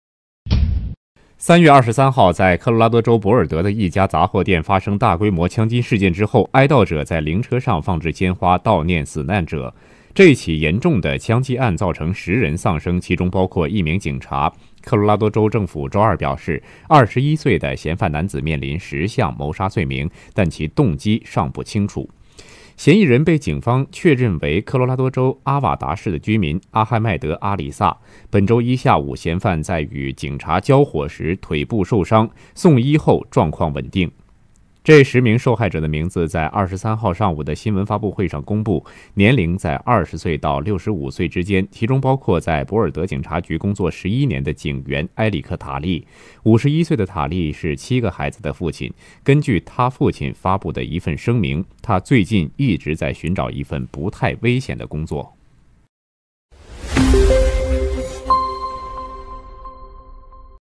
科州殉职警员父亲：他曾想换份安全的工作
科罗拉多州枪击案受害者的名字在23日上午的新闻发布会上公布，年龄在20岁至65岁之间，其中包括在博尔德警察局工作11年的警员埃里克·塔利。 51岁的塔利（Talley）是七个孩子的父亲，根据他父亲发布的一份声明，他最近一直在寻找一份不太危险的工作。